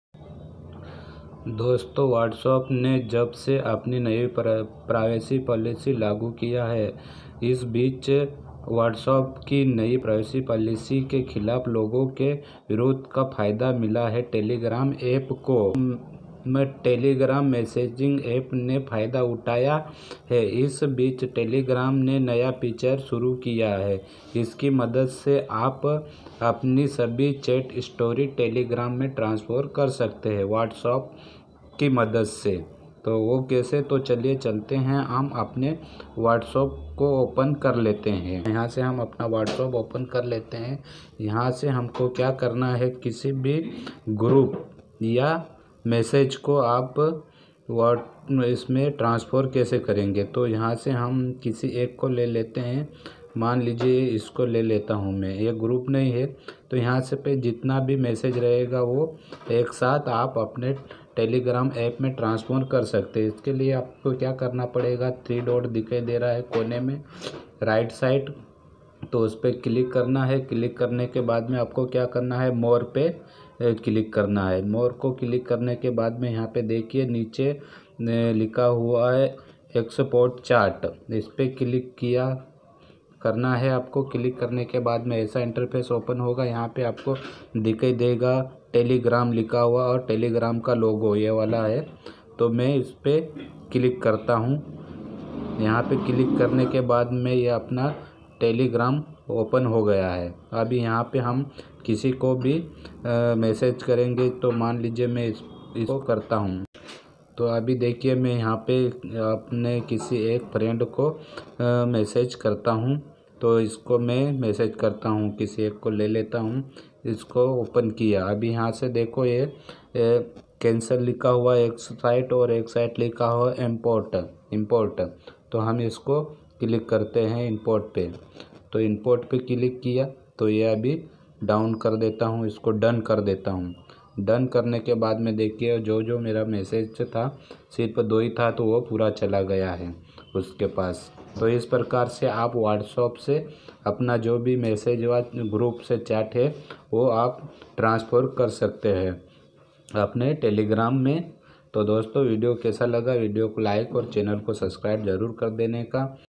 WhatsApp new update व्हाट्सएप न्यू प्राइवेसी पॉलिसी अपडेट
doston is video mein maine aapko bataya hai ki WhatsApp ne apni new privacy policy lagu Kiya hai aur WhatsApp ki sabhi chat history telegram mein transfer kar sakte hain WhatsApp ki madad se chat ya group ko transfer kar sakte hain WhatsApp se WhatsApp ke chat group ko type karke yaa chat group massage ko